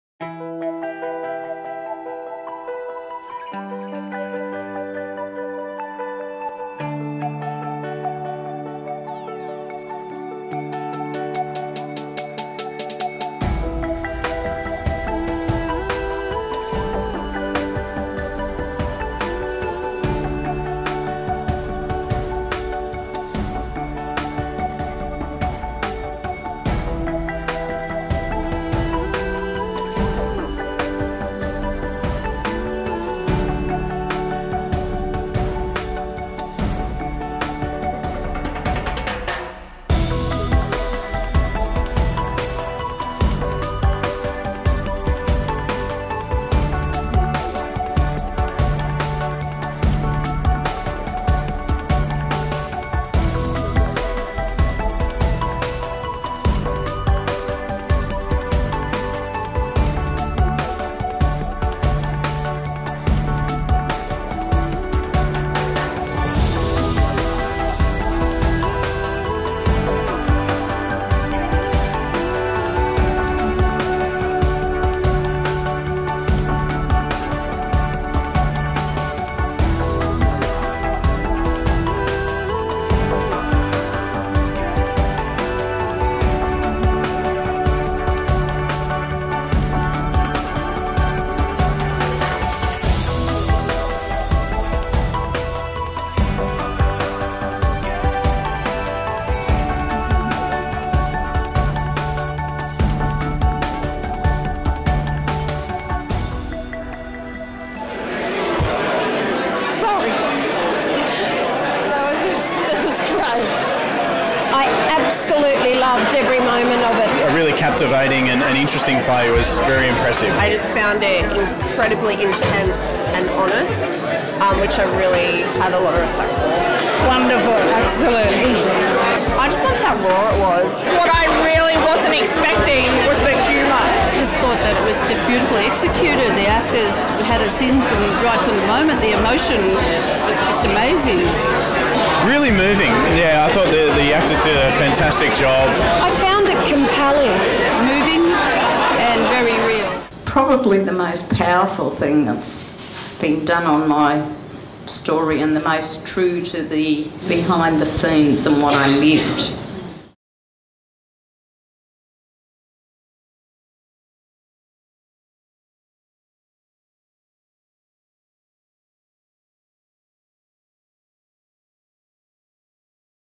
Letters to Lindy
Filled with humour and heartbreak, this stunning work explores the public’s relationship with one of 20th Century Australia’s most iconic figures.